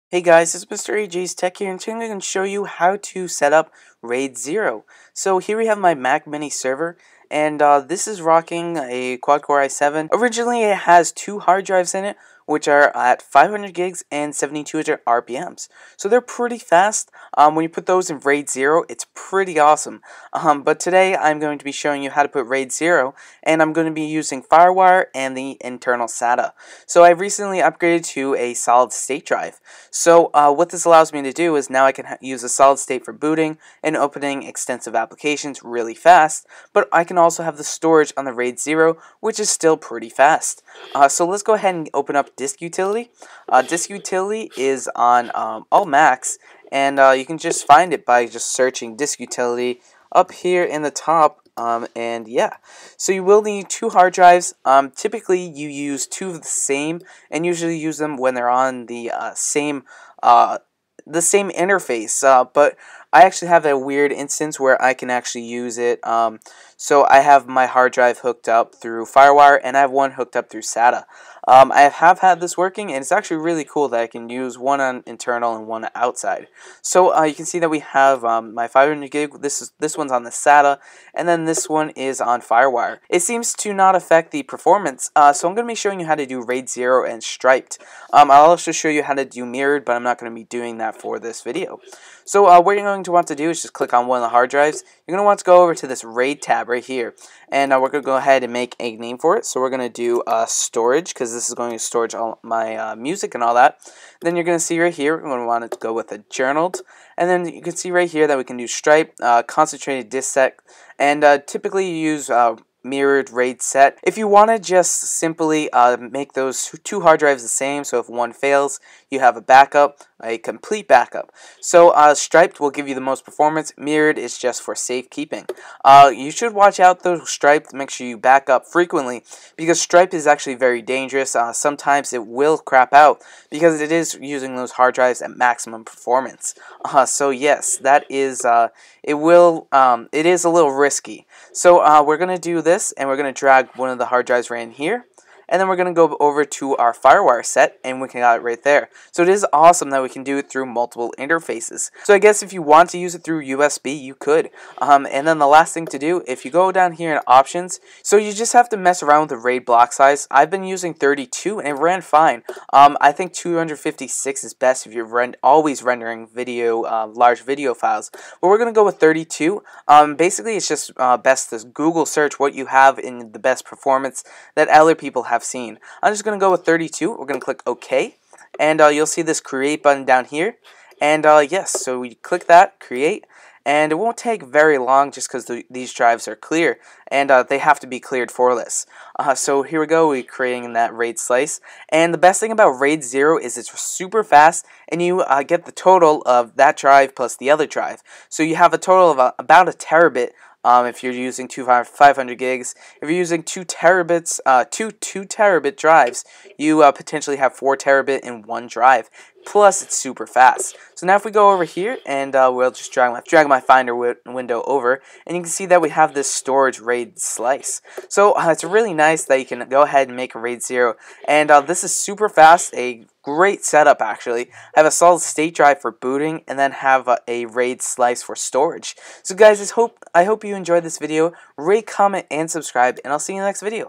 How to setup RAID on Mac 2012
In this video I show you how to setup a RAID-0 (stripped) or RAID-1 (mirrored) configuration. RAID is a more advanced user setup. Typically users use RAID-0 for more performance and RAID-1 for reliability. Mirrored means the hard drives are complete copy of themselves that we if one goes there is a complete copy of the contents. With RAID-0 or stripped you can maximize your performance. RAID-0 is my preferable configuration because it is fast and doubles storage. It is fast because it rights half of a file to one drive and half to the other therefore maximizing the performance of the two drives.